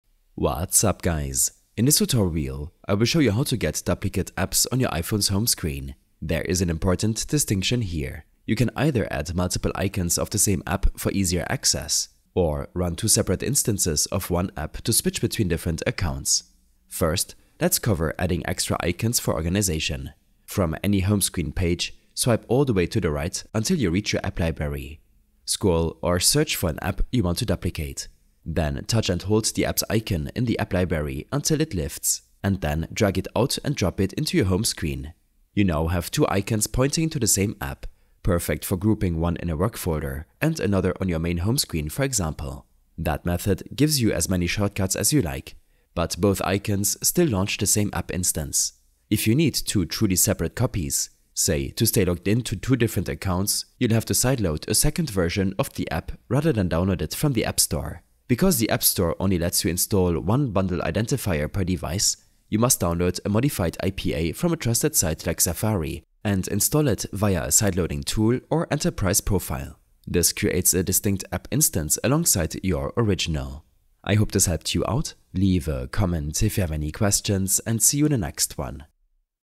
How To Have Duplicate Apps on your iPhone
A short tutorial on getting duplicate apps on your iPhone’s home screen.